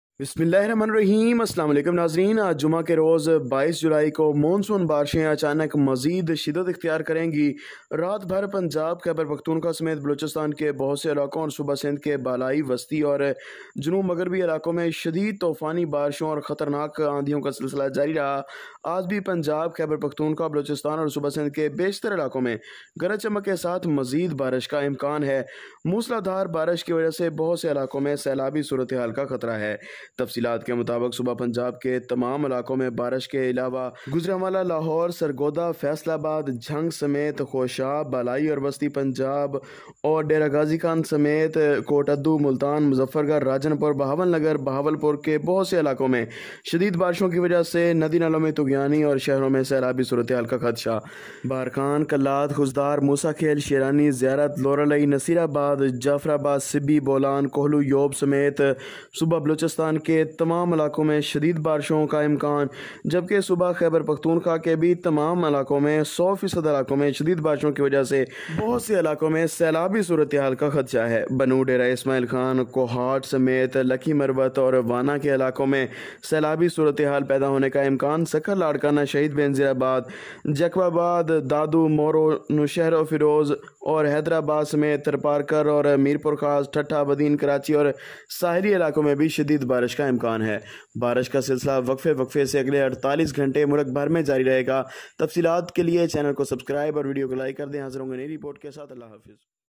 More rains are coming tonight | 22 July Friday | Heavy Rains and windstorm expected | Weather report
In this video details
Monsoon Rains are expected from 30th June
Met Office informed that moist currents from Arabian Sea and Bay of Bengal are likely to enter in upper parts of the country from 29th June (Wednesday) which may intensify and expand to southern parts of the country by the end of this week. Under the influence of this, the expected weather will be as under:

Rain-windstorm/thundershower (Isolated heavy falls) are expected in Karachi, Hyderabad, Thatta, Badin, Dadu, Zhob, Ziarat, Barkhan, Loralai, Kohlu, Quetta, Kalat, Khuzdar, Lasbella, Naseerabad and Sibbi from 1st July to 05th July.
Possible Impacts:
Heavy falls may generate urban flooding in Islamabad, Rawalpindi, Lahore, Gujranwala, Sialkot, Faisalabad from 02nd to 04th July & in Karachi and Hyderabad from 03rd to 05th July.
Fishermen are advised to remain careful due to rough sea conditions from 03rd to 05th July.
Flash flooding is also expected in local nullahs of Kashmir, Khuzdar, Lasbella, Awaran, Barkhan and Kohlu during the forecast period.
Heavy rainfall may trigger landslides in Kashmir, Galliyat and Murree.
The rising temperatures are likely to subside during the forecast period. Windstorms may damage loose structures at vulnerable locations.
Rainwater will be beneficial for the sowing of rice crop.
Travelers and tourists are advised to remain extra cautious during the forecast period.
NOTE: All concerned authorities are advised to remain 'ALERT' during the forecast period